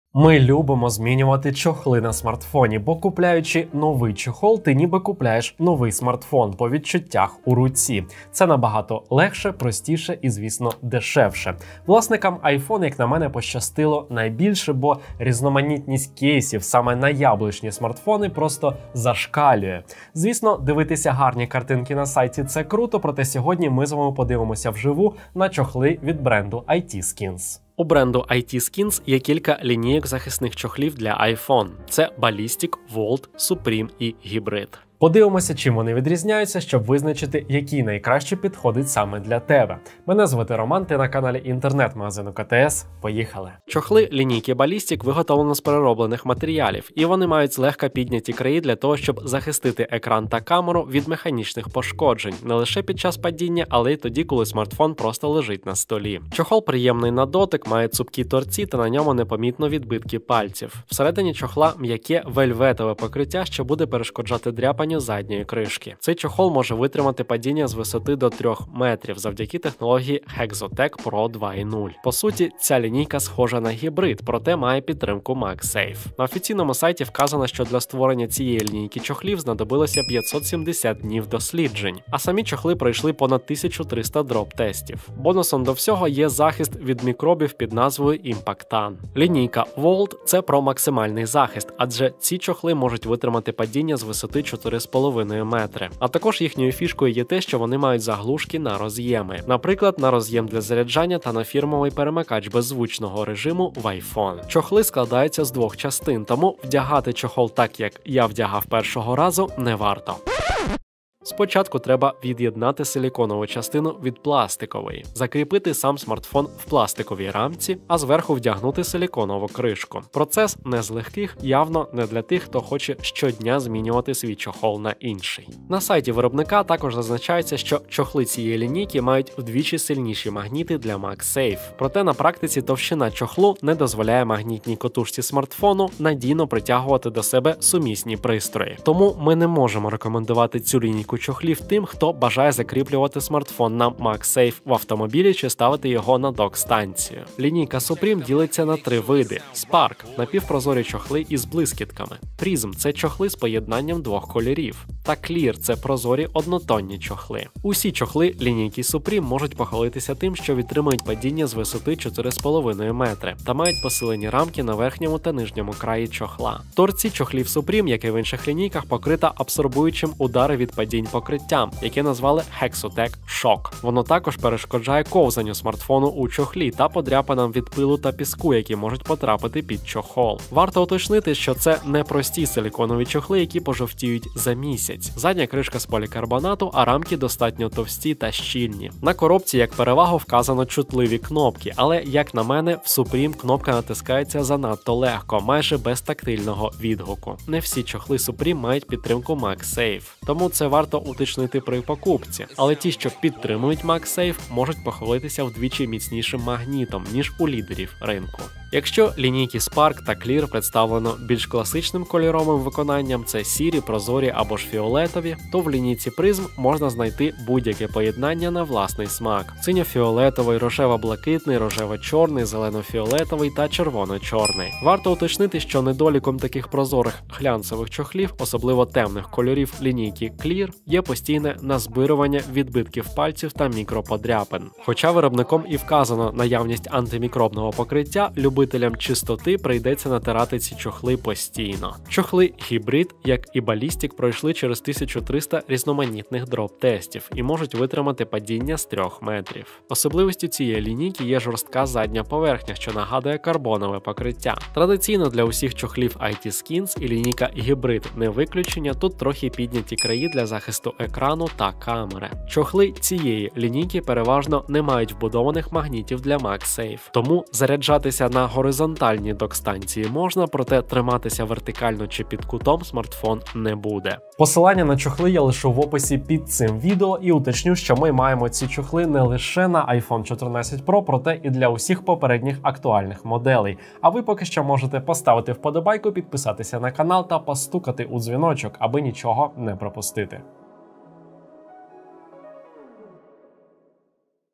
Чохли iTSkins з MagSafe для iPhone
Чохли iTSkins BALLISTIC, VAULT, SUPREME, HYBRID з MagSafe для iPhone
00:00 - Посилання в описі
00:43 - BALLISTIC
01:28 - VAULT
02:27 - SUPREME
04:01 - HYBRID